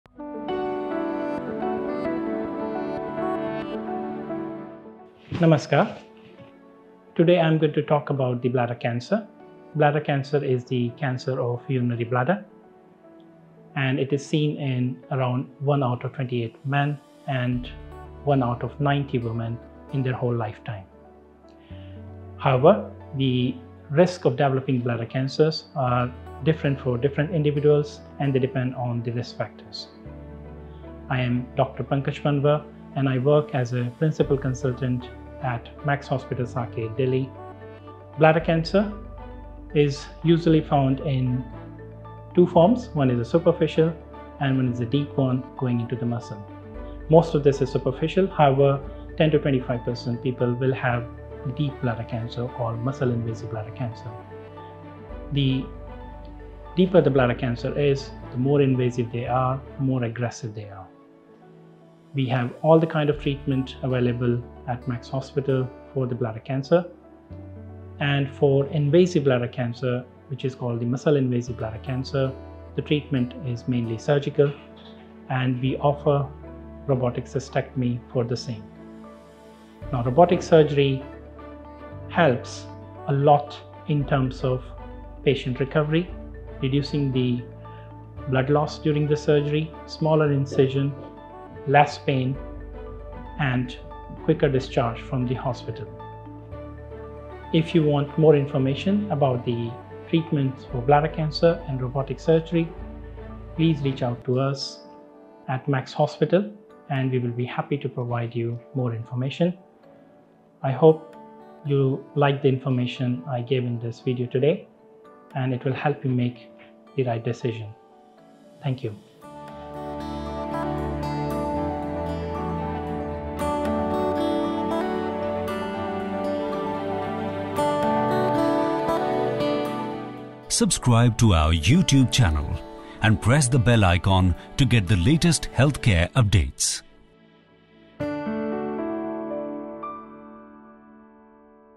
Robotic Surgery for Bladder Cancer | Dr. Pankaj Panwar | Max Hospital, Saket
Bladder cancer refers to the cancer of the urinary bladder. It is seen in 1 out of 28 men and 1 out of 90 women.  Dr. Pankaj Panwar Principal Consultant - Uro-Oncology and Robotic Surgery Max Super Speciality Hospital, Saket talks about bladder cancer and robotic surgery as a treatment for it.